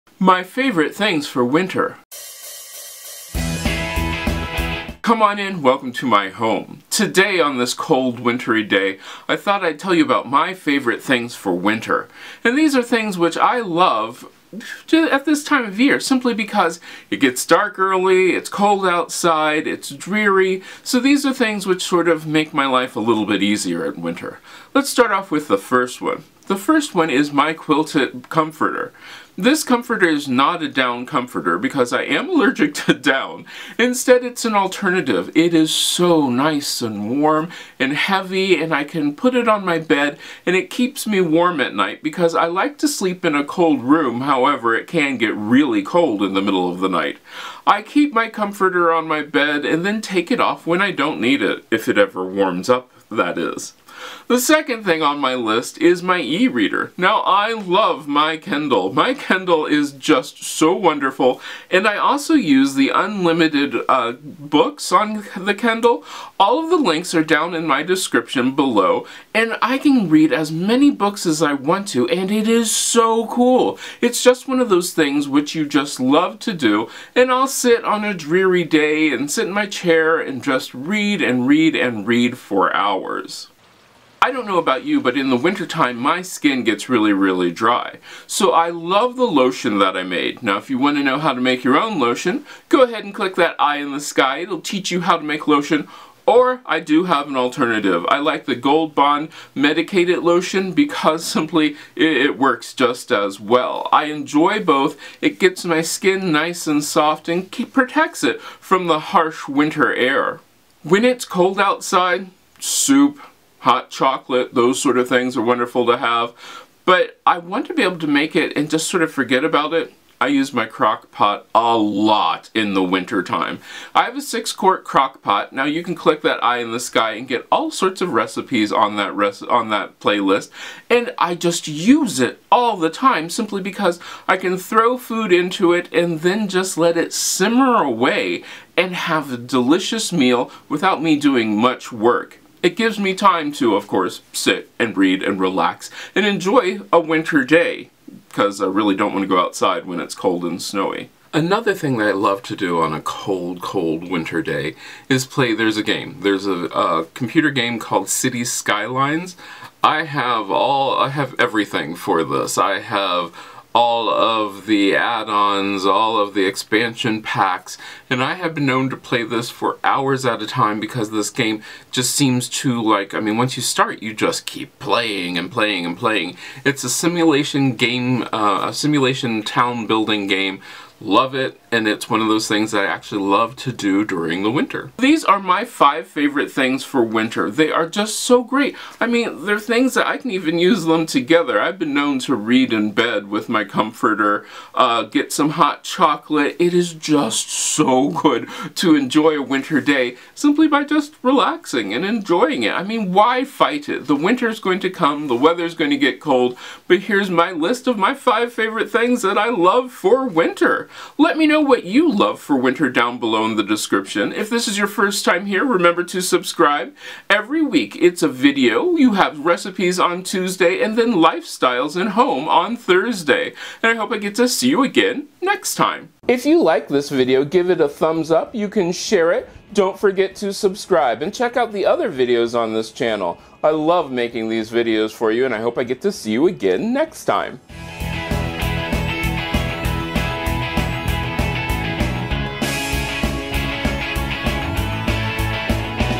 My Favorite Things for Winter 2019
It is cold and dark outside so I thought what better way to make the days a little lighter than to share with you the things that I love for winter.

My Favorite Things
Comforter – Queen

Kindle Paperwhite

Kindle unlimited

Gold bond lotion

Cities Skylines

Special Thank You For Your Support
MJ
Sal

RECOMMENDED PRODUCTS AND GEAR